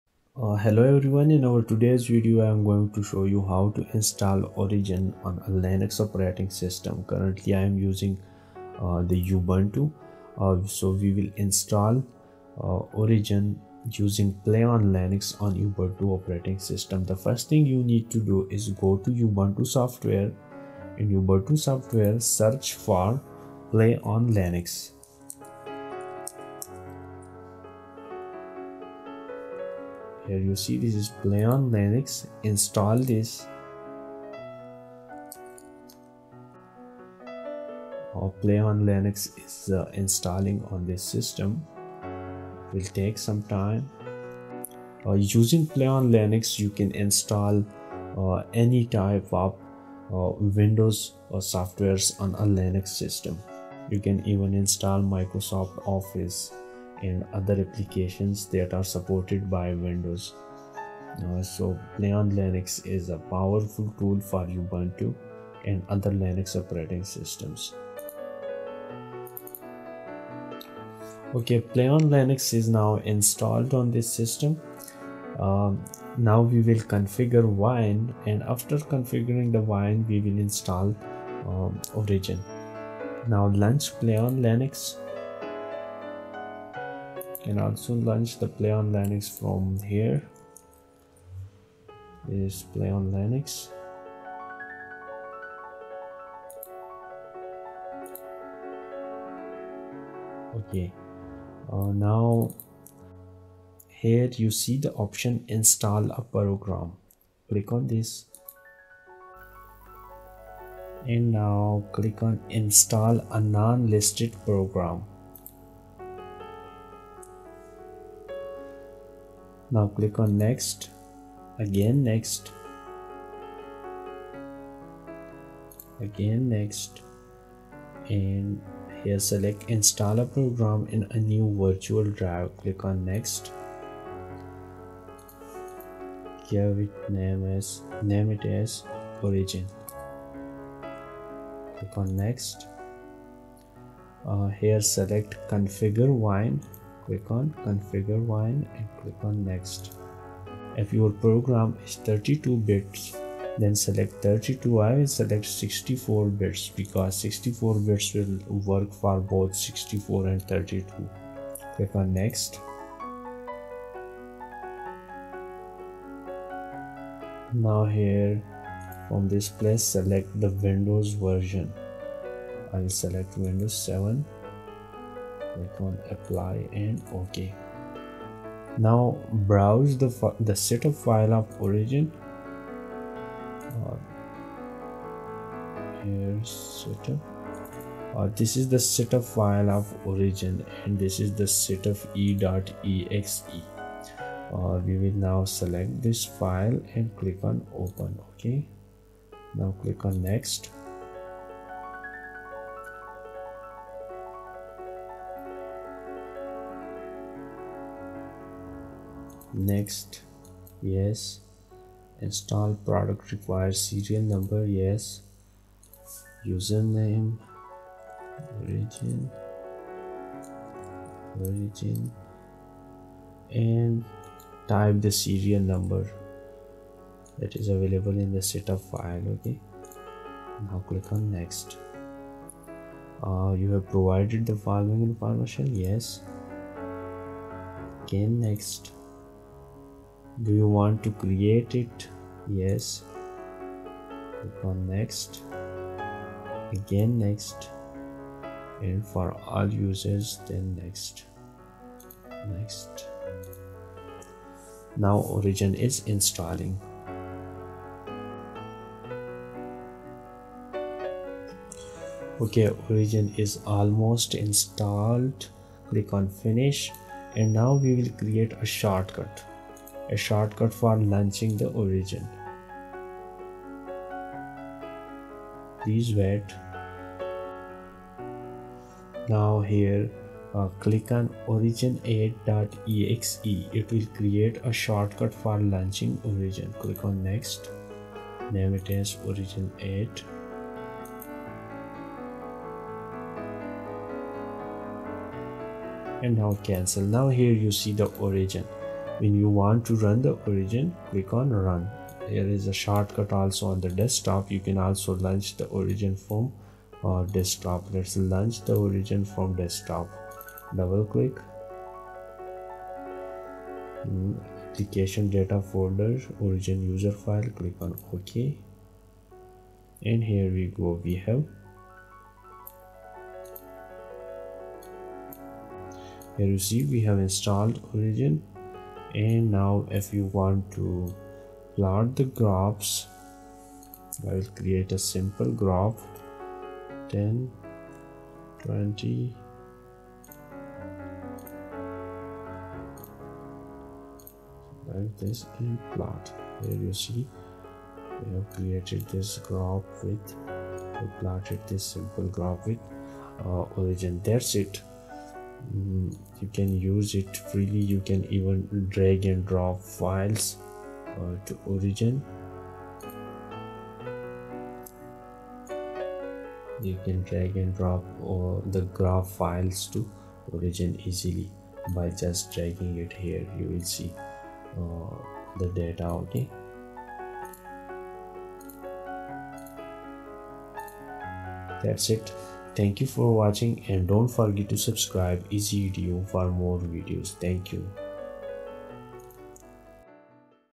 How to install origin and windows application on ubuntu (linux) using playonlinux (updated video)
Origin is graph plotting software used by researchers for graphing and analysis of results in the form of graphs. It can be installed on linux and ubuntu operating

systems. In this video we demonstrated how use origin and install it on a linux or ubuntu system.
Procedure and steps
1. you must have a valid setup file for origin
2. you must a good internet connection
Steps
1. install playonlinux that is availabe in ubuntu
2. setup the wine version for play on linux
3. click on install unlisted software
follow the remaining procedure in the video it's really simple and fast
Now you don't need to switch to windows for plotting graphs. you can do all your work in origin while staying on ubuntu or linux
This procedure is valid for all linux operating systems like linux, linux mint, ubuntu, and other debine version